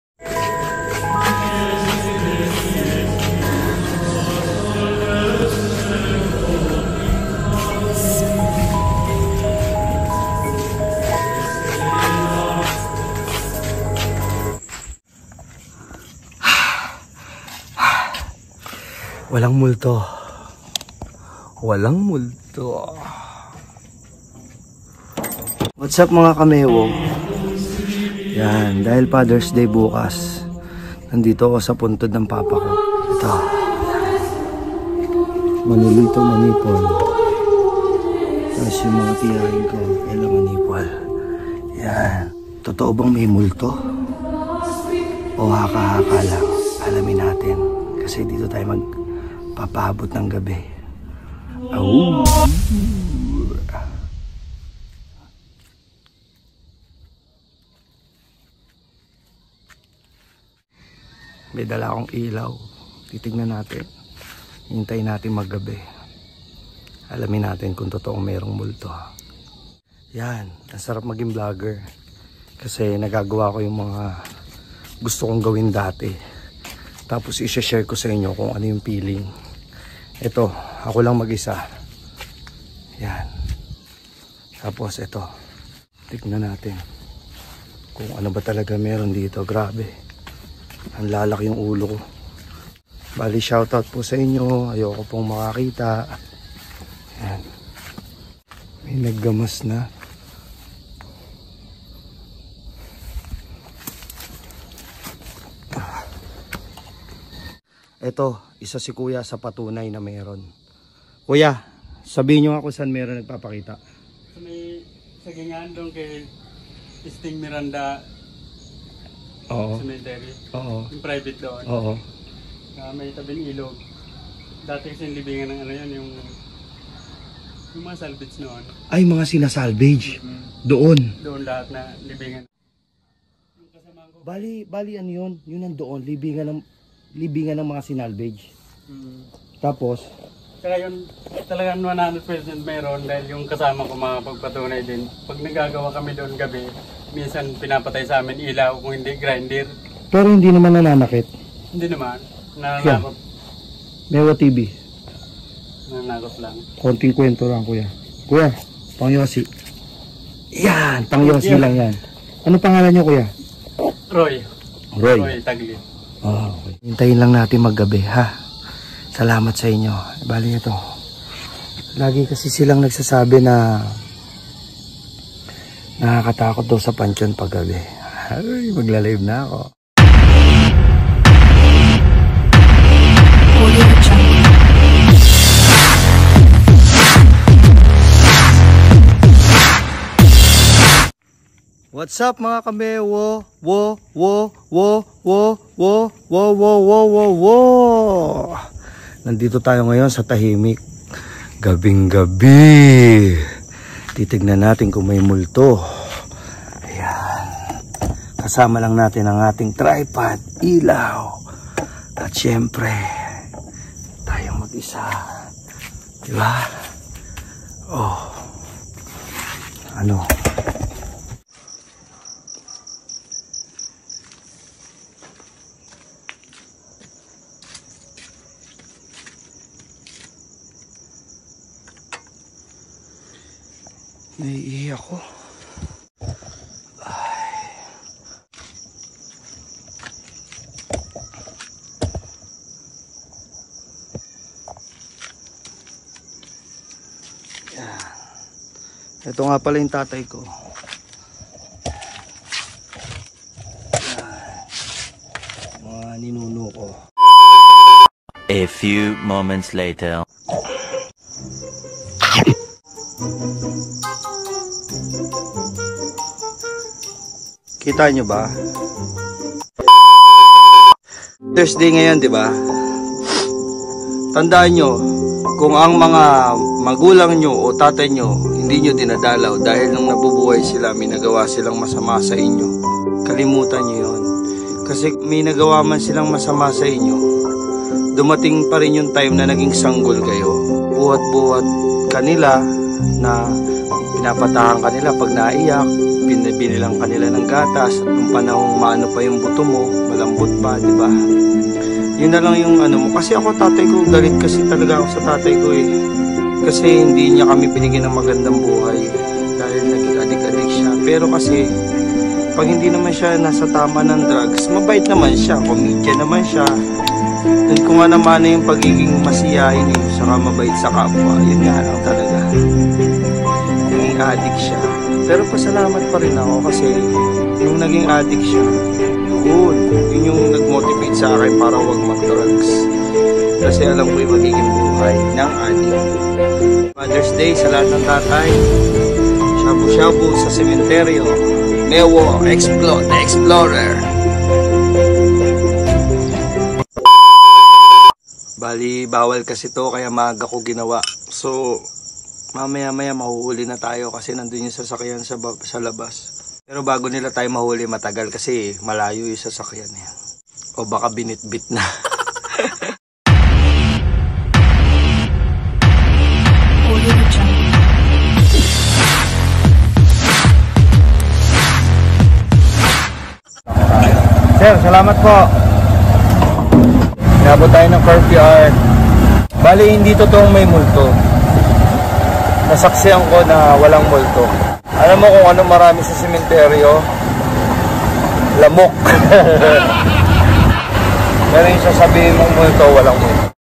Multo totoo ba? Mukbang sa sementeryo
Father’s day special po tayo sa sementeryo

salamat at naranasan kong lagpasan ang takot ko
Sa sementeryo pag gabe

promise walang multo

Likha lang ito ng imahinasyon natin

kase kung kaya nilang bumalik
Galing sa kabilang buhay
Wala sanang malungkot ngayon

Salamat mga kamewo marame p tyo mga videos hehehe sobrang saya maging vlogger